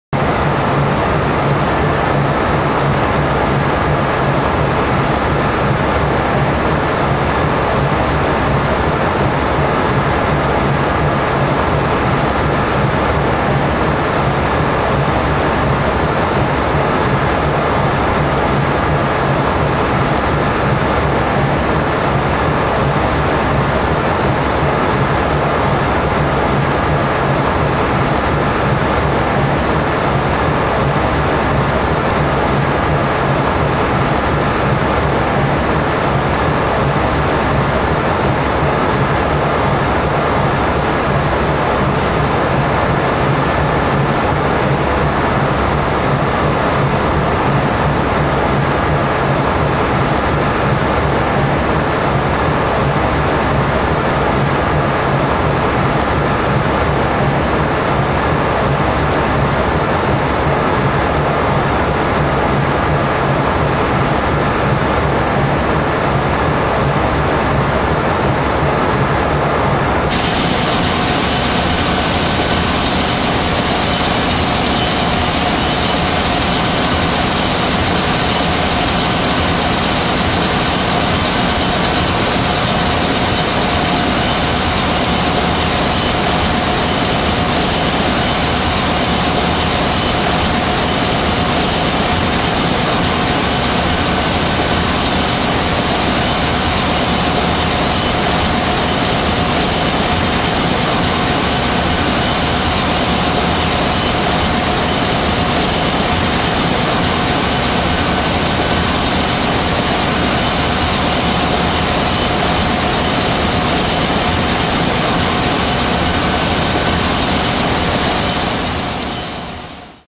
Khaitan Sugar Mill, West Bengal, India 2006
Khaitan Sugar Mill is near Plassey, famous for a battle where Clive secured Bengal for the British. In 2006, we found some surviving stationary steam here. These are Stork vacuum pumps, the first is 7377/1956, the other similar but does not have a number on its plate . Other clips show the Stork mill engine and Bellis and Morcom generators.